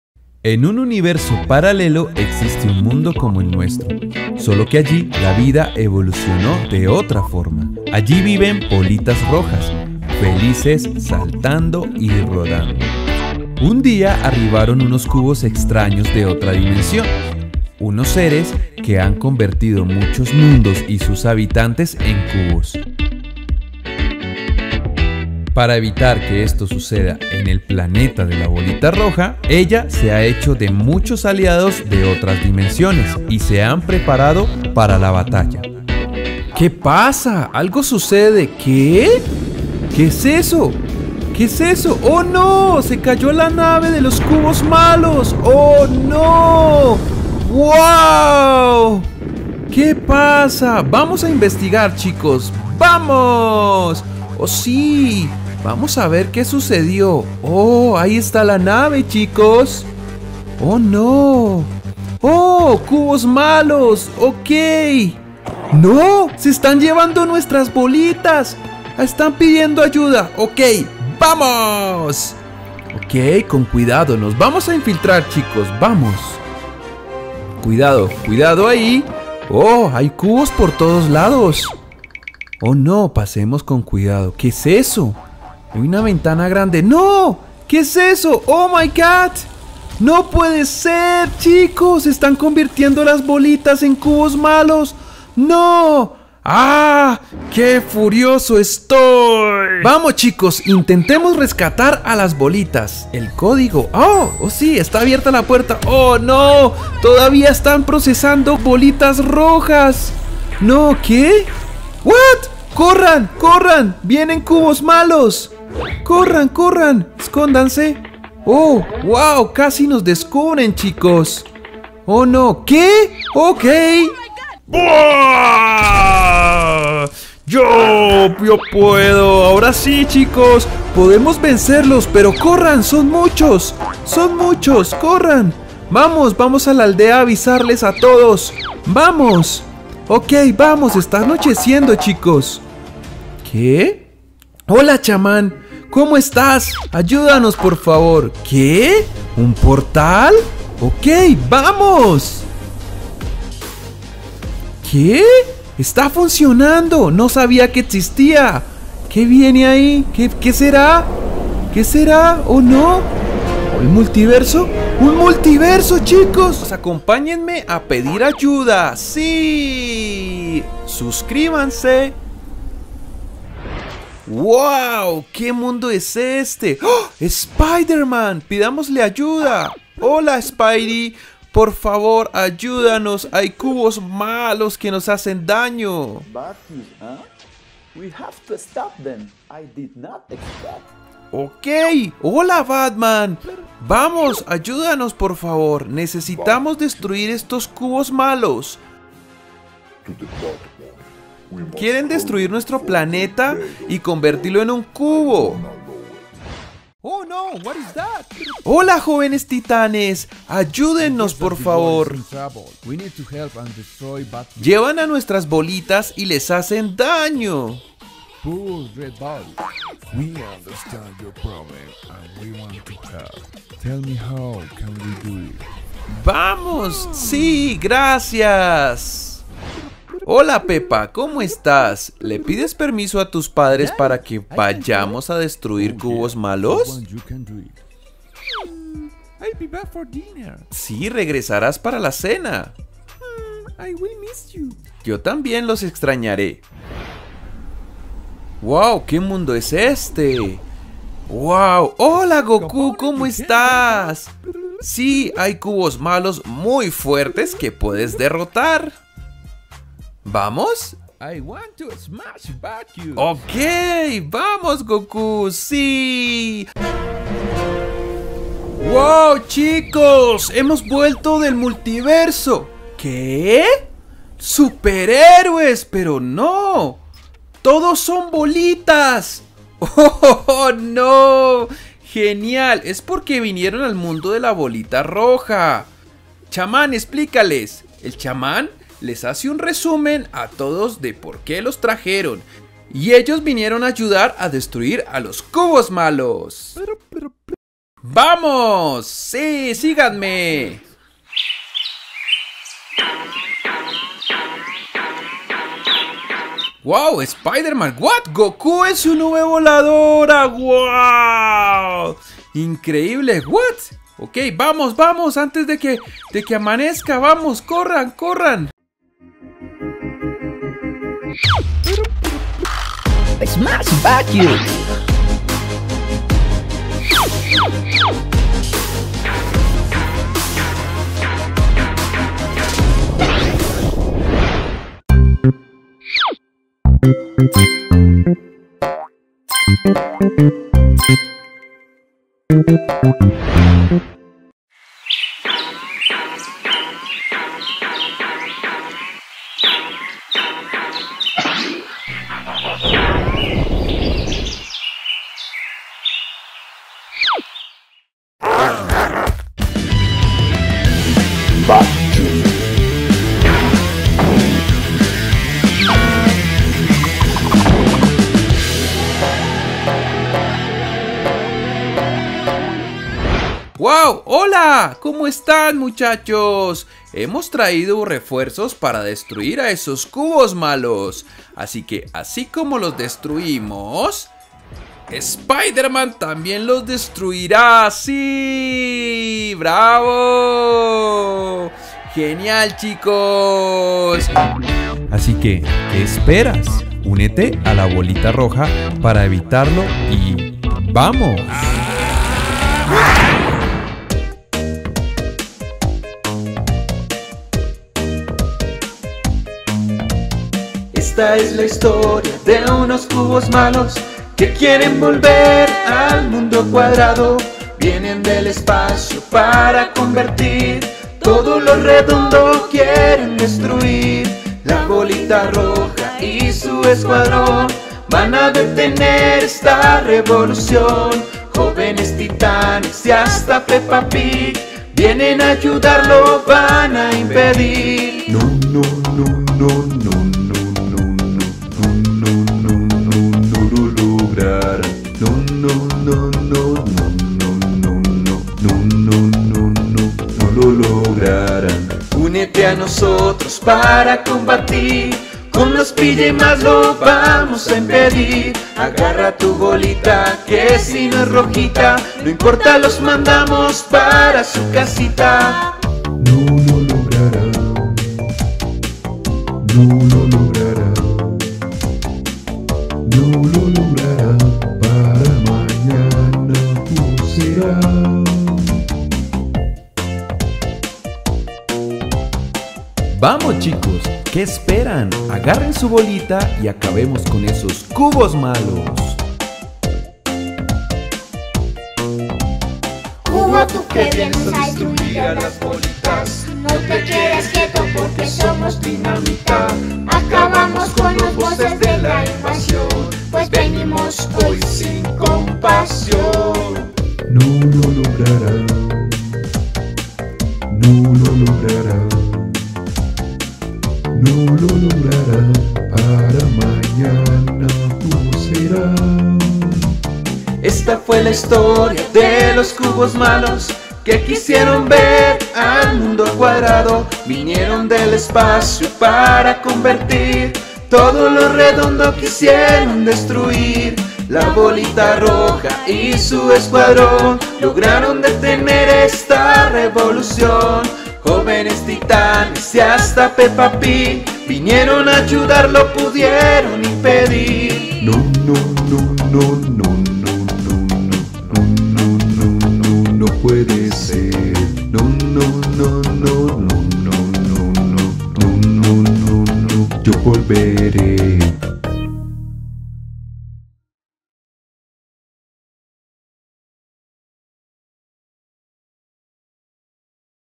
El ORIGEN de la Bolita Pro 🤯 😎
Les presento la HISTORIA de la Bolita Roja y cómo se convirtió en la Bolita Pro. Diviértanse. 🤩 😂